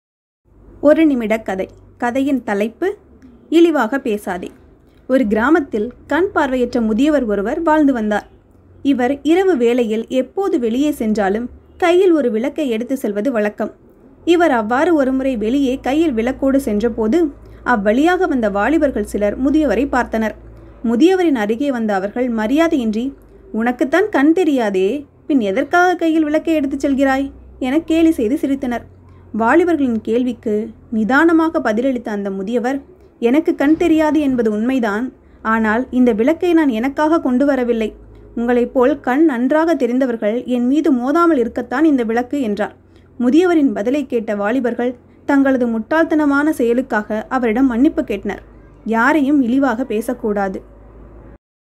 ஒரு நிமிடக் கதை 7 | இழிவாக பேசாதே | one minute story in tamil
1 minute story in tamil | இழிவாக பேசாதே
ilivaga pesathe

@VIP Talks Mrs.Vimala